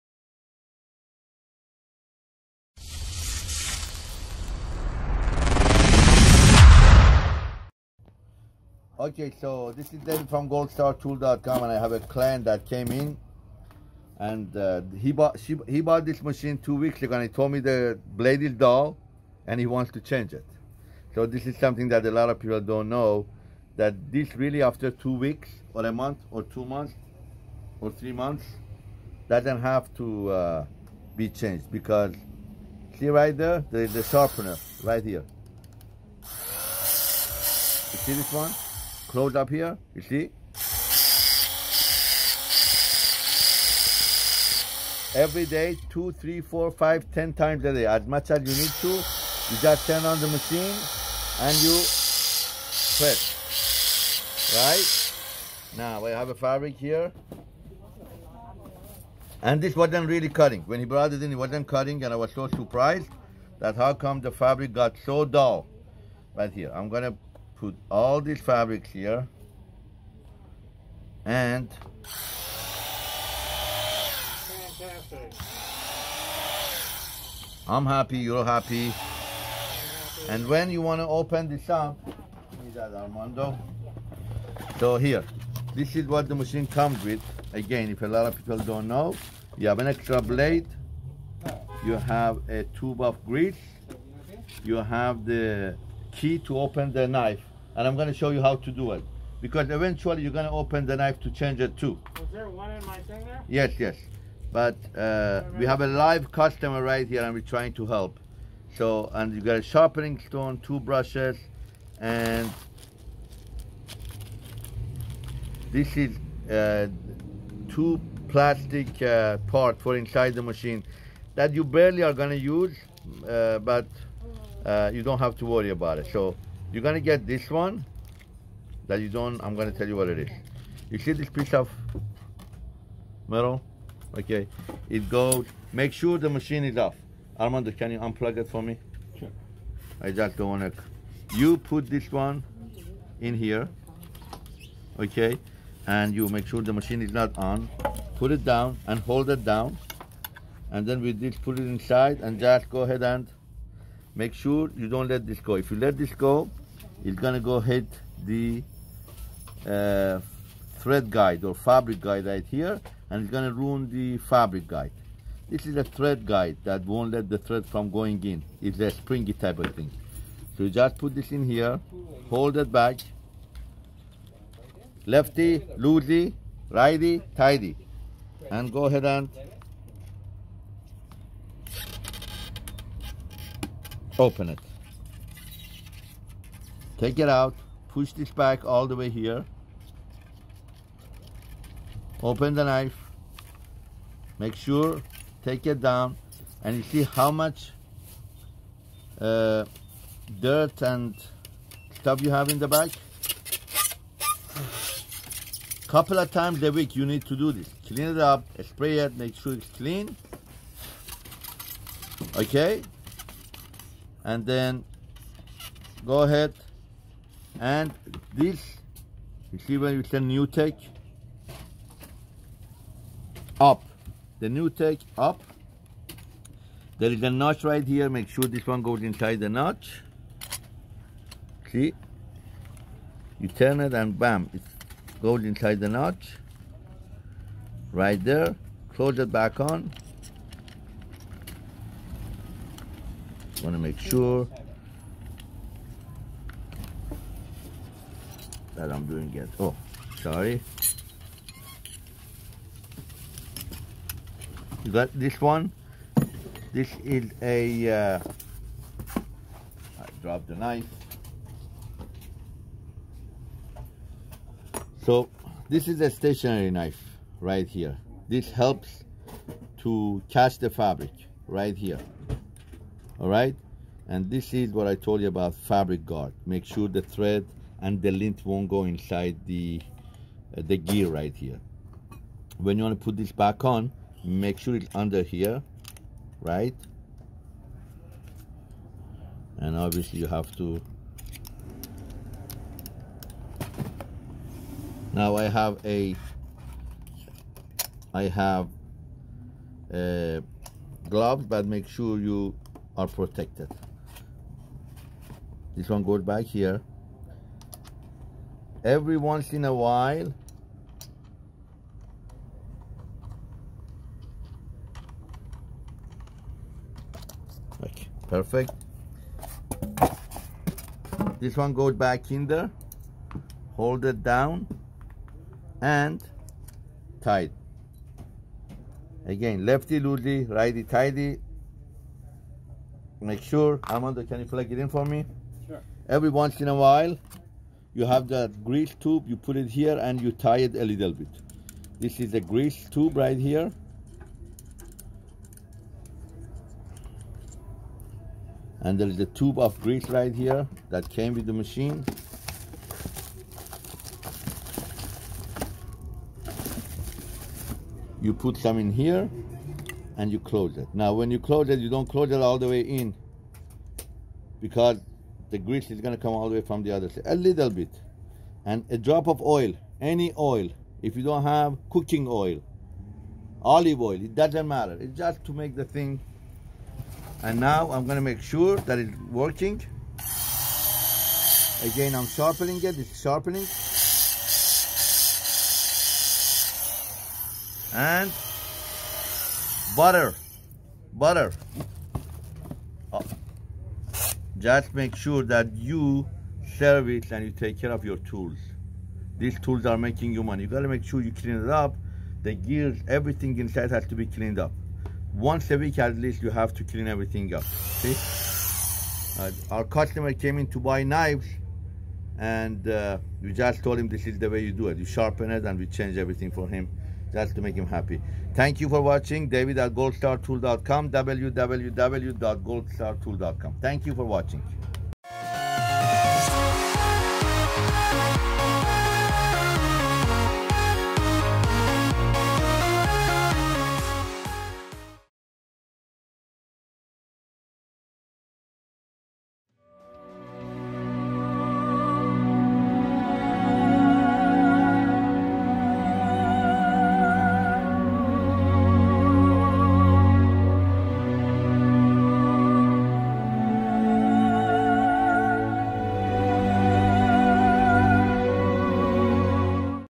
Tutorial - Maintaining Your New-Tech Rotary Cutter - Goldstartool.com
Today, David shows us how to sharpen and care for your New-Tech 3-inch rotary cutter.